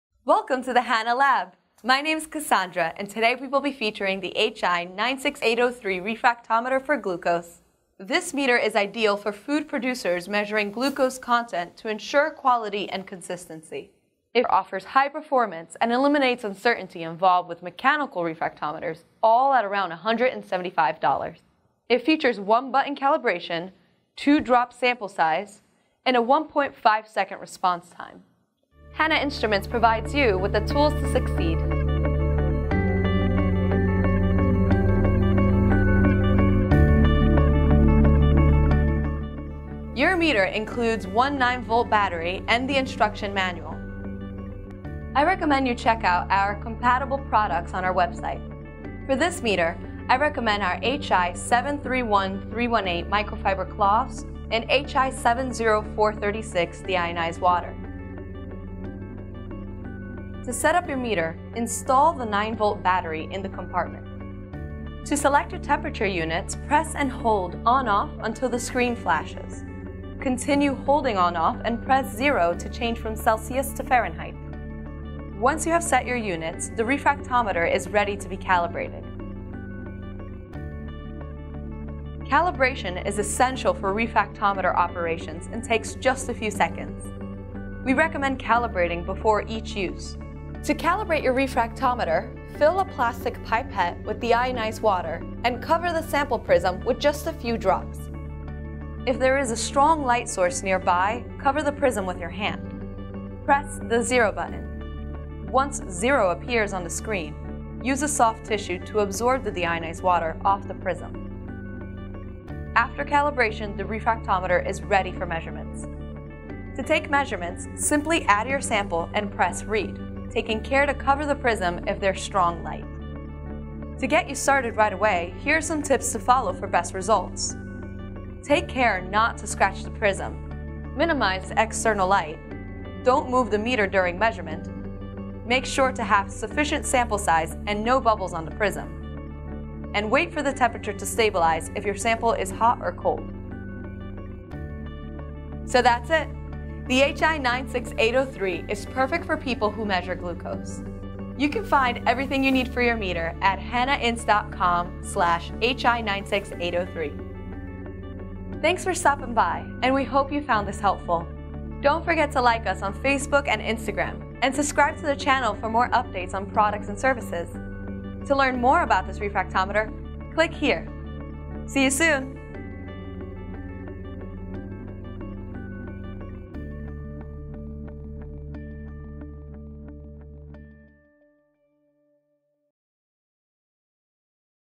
Hanna Lab - Learn About the Hanna Instruments Glucose Refractometer HI96803
The HI96803 is a rugged, portable digital refractometer for food products designed to report the content of aqueous solutions containing sugar as % glucose by weight (% w/w). The HI96803 report the results with an accuracy of ±0.2%. The operation of the meter is simplified with only two buttons: one button is to calibrate with distilled or deionized water and the other to take a measurement. All readings are automatically compensated for temperature variations according to the ICUMSA Methods Book standard and displayed within a 1.5 second response time. The sealed flint glass prism and stainless steel well are easy to clean. Just wipe with a soft cloth in preparation for the next sample.